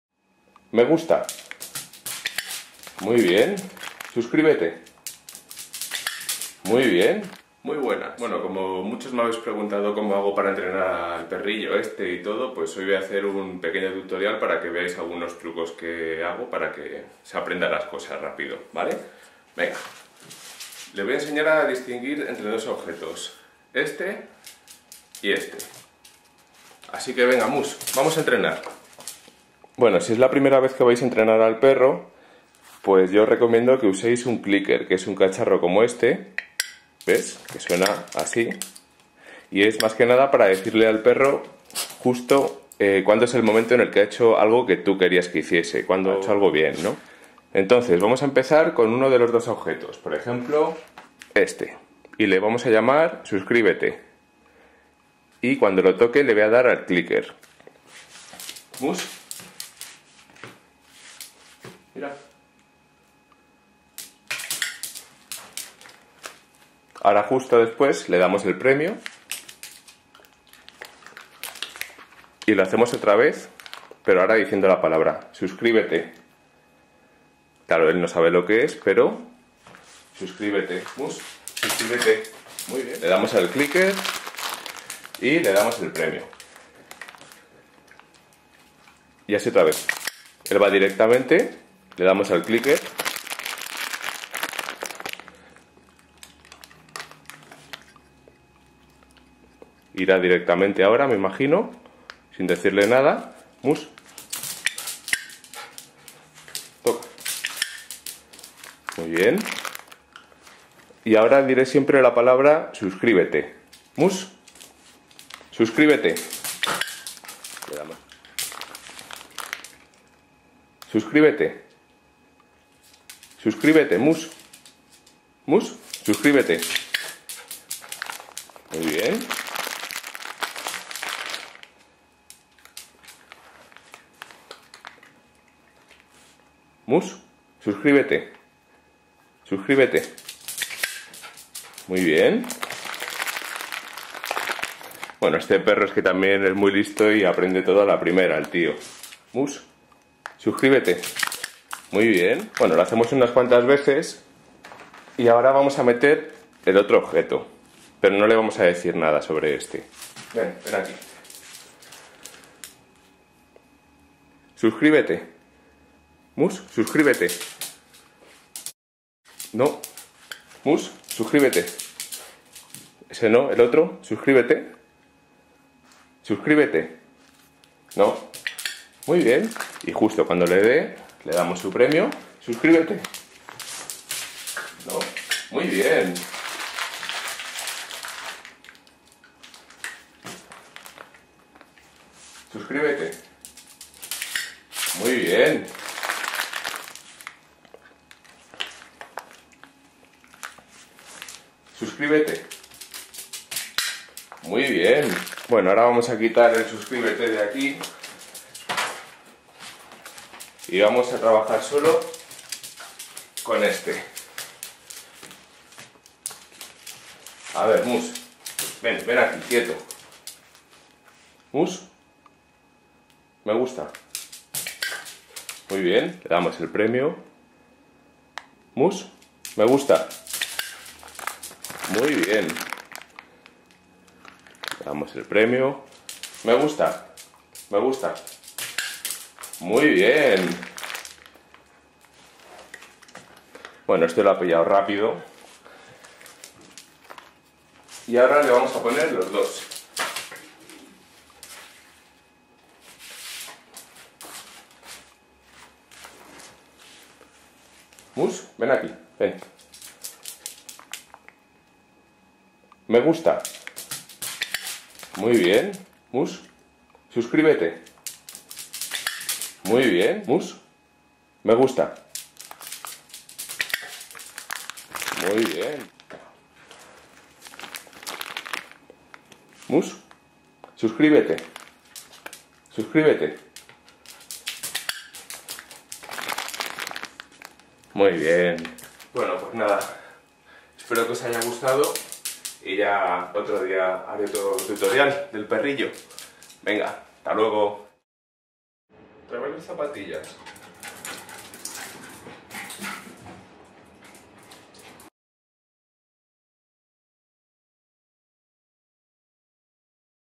Perro que vota y se suscribe - Cómo entrenar a un perro
Muchos de vosotros me habéis pedido que explique cómo enseño a nuestro perro Mus, pues bien, aquí tenéis este tutorial con el que podréis aprender a entrenar a vuestro perro. Aquí utilizo un clicker para enseñarle a diferenciar dos objetos. Espero que os guste!!